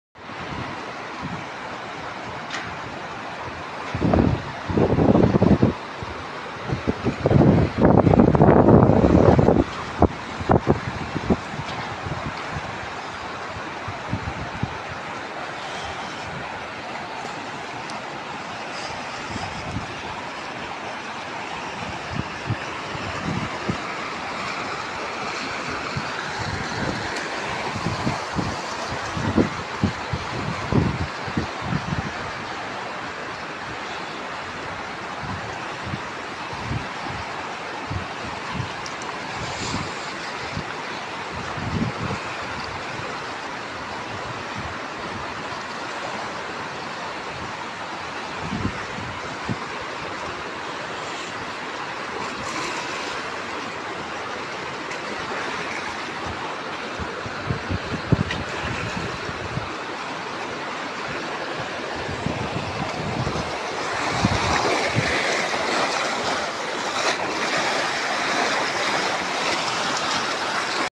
واد زيز 🌊 الحمد لله
الحمد لله و الشكر لله 🤲🤲 حمولة واد زيز يوم : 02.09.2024. و الحمولة في طريقها إلى سد الحسن الداخل بالرشيدية.
الأمطار  الرعدية ⛈️🌩️🌧️  هي من أدت إلى هذه الحمولة المهمة. المياه ستعود إن شاء الله إلى مجاريها و كذلك وادي زيز ييعود لحياته الطبيعية.